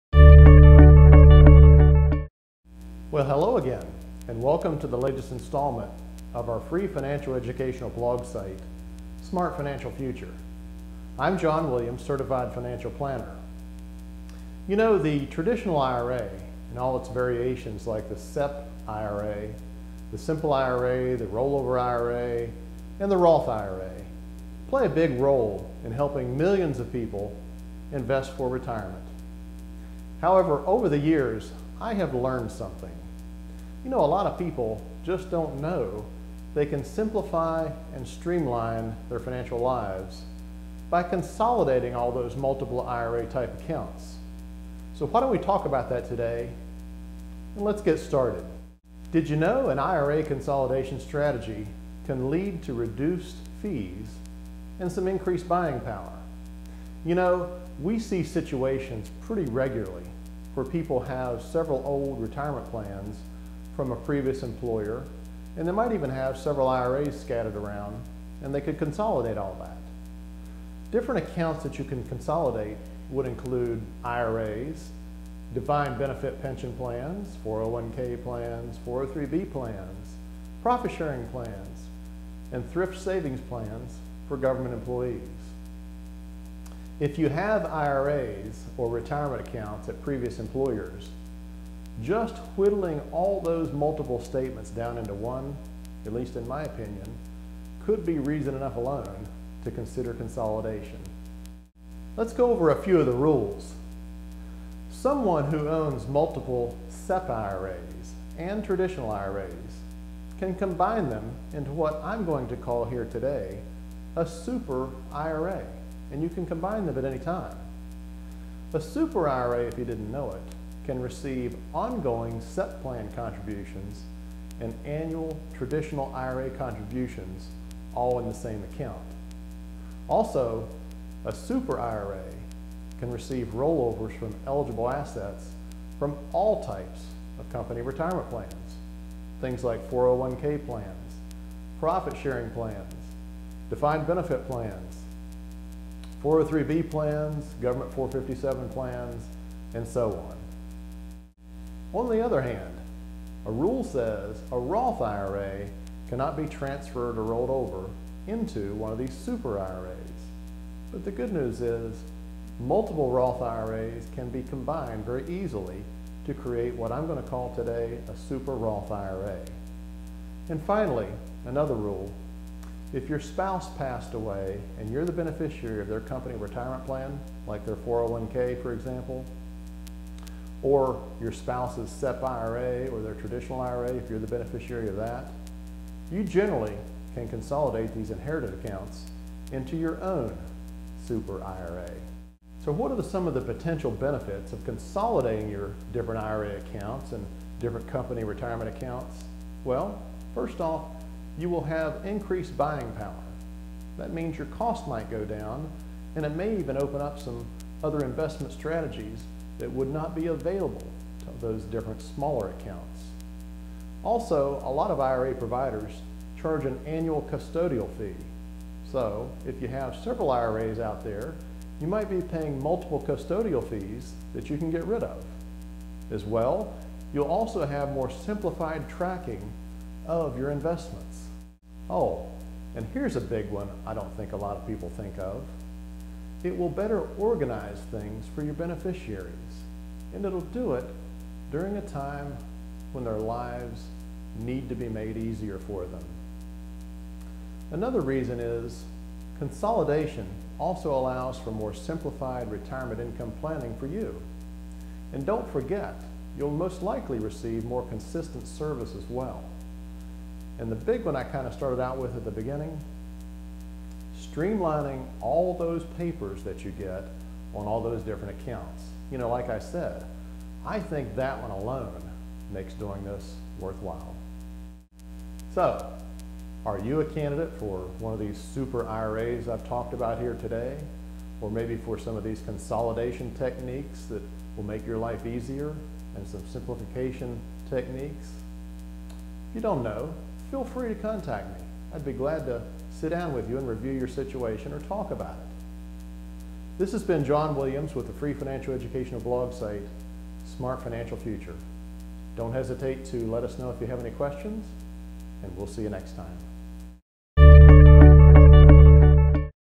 IRA Consolidation | Financial Planner in WV
Consolidation strategies for IRAs, 401k, SEP IRAs and other retirement accounts.  By John D Williams, Founder and Senior Advisor at Ironwood Wealth Management in Teays Valley WV.